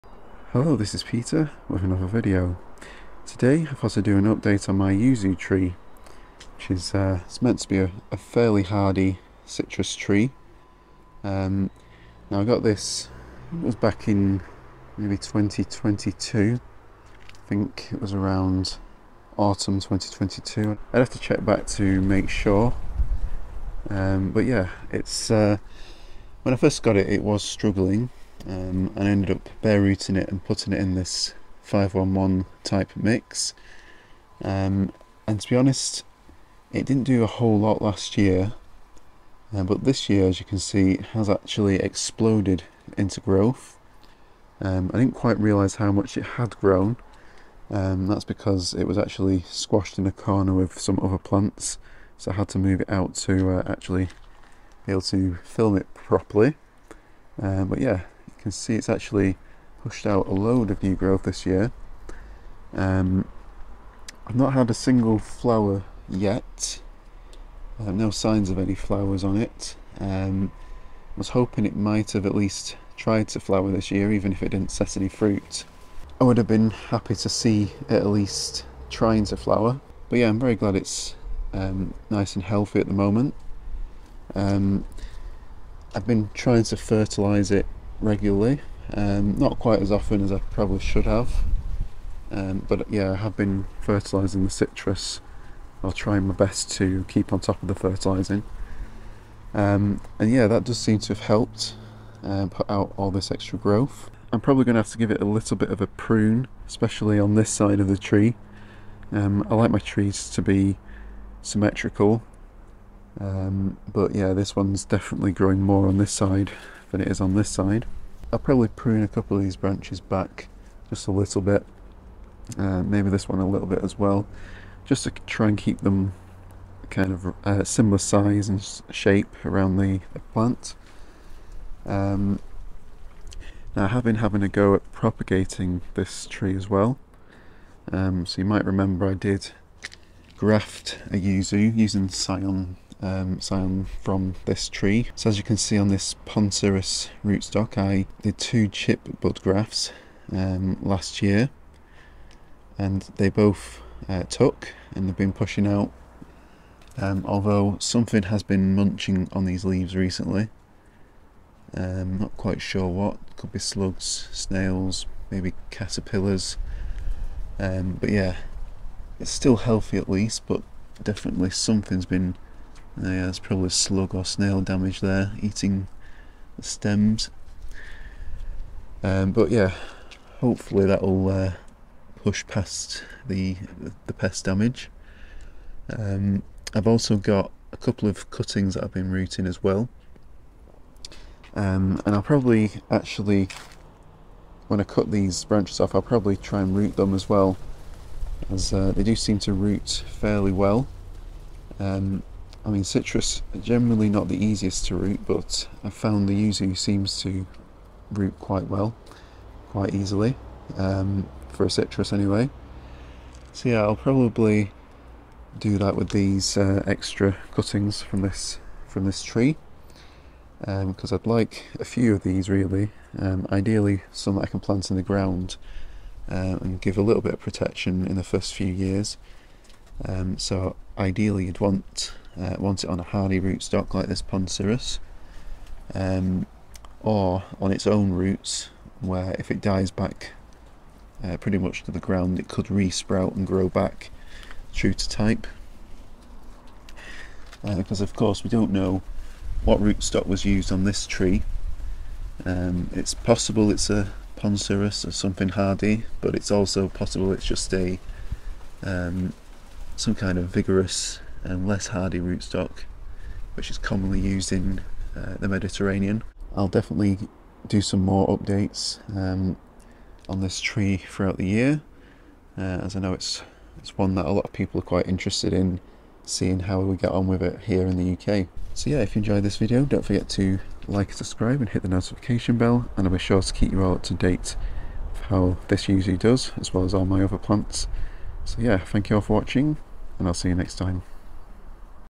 Yuzu Tree Update (Citrus Junos) - July 2024
In this video, I give a quick update on how my Yuzu has been doing now we are in mid-summer. There has been a lot of growth since the last update. I also show my other Yuzu tree I grafted myself last year. The Yuzu (Citrus x junos) is a hardy citrus tree that should be able to survive colder temperatures than most varieties.